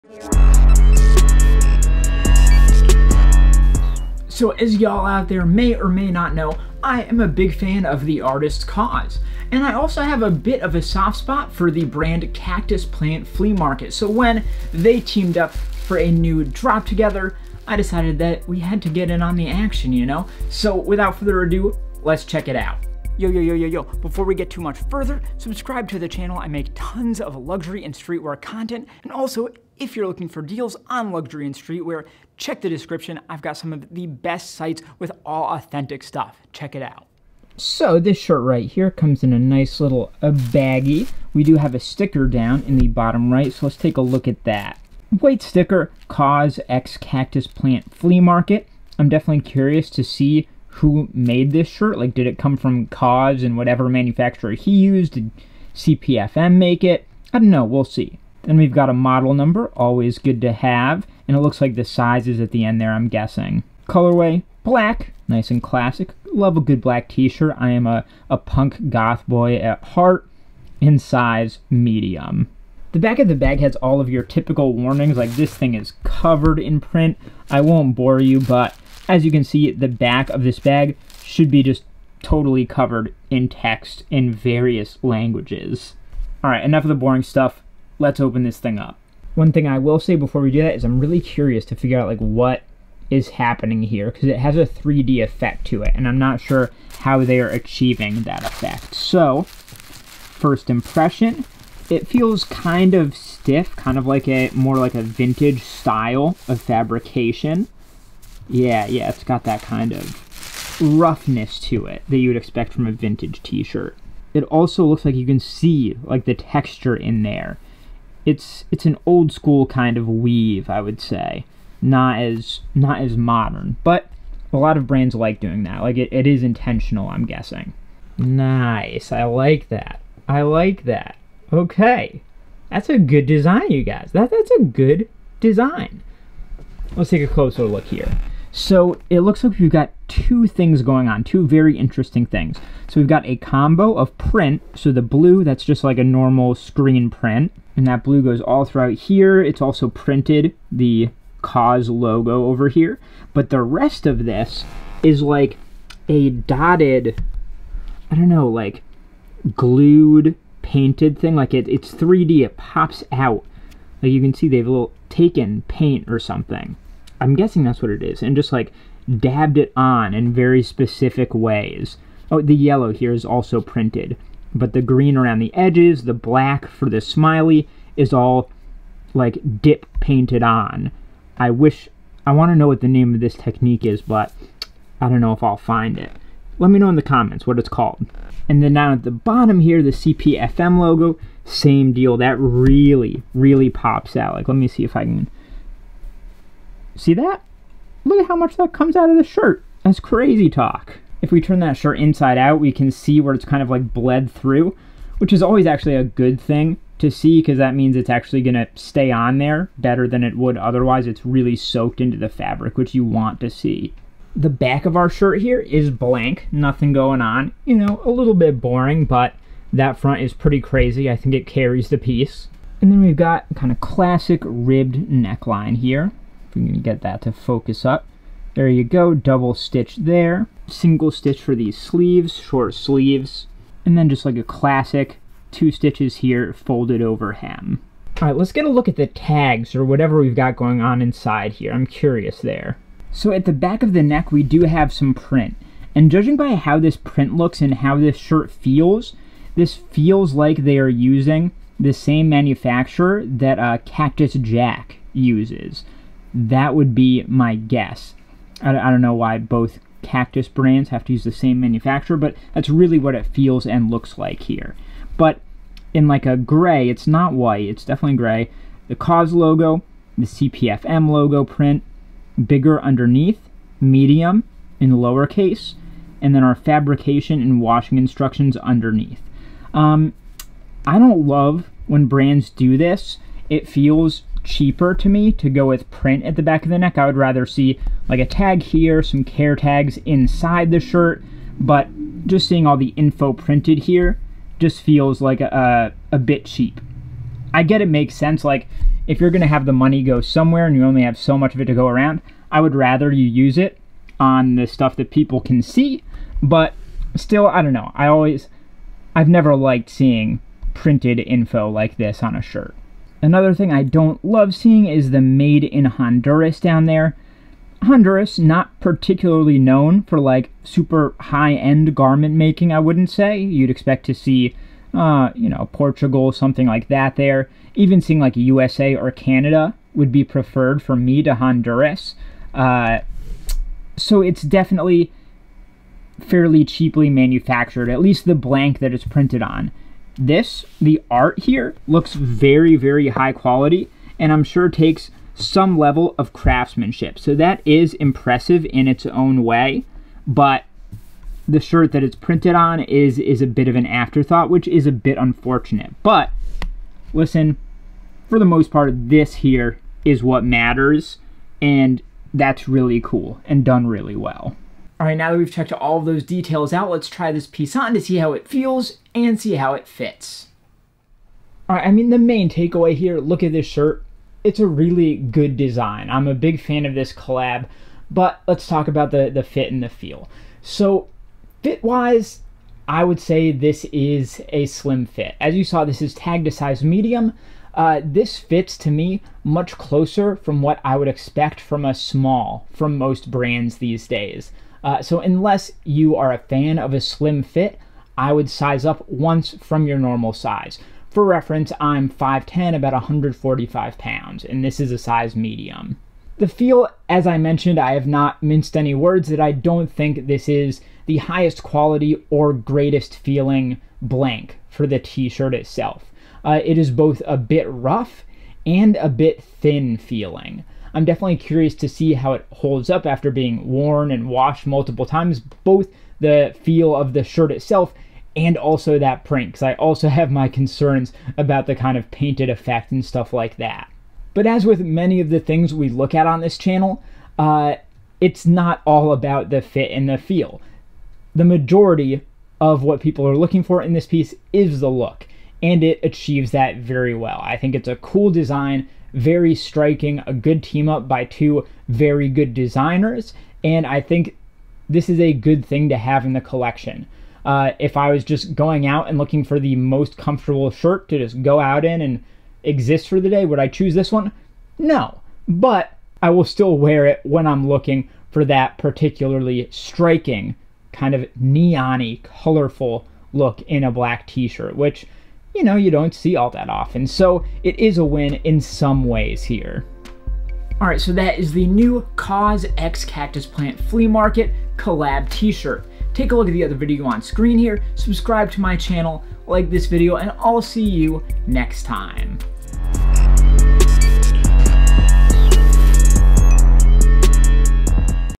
Real KAWS x Cactus Plant Flea Market t-shirt | Legit check, unboxing & fit review
00:00 Introduction
00:37 Bag
01:54 Close-Up Look
03:58 Neck Tag
06:04 Try On
08:00 Outro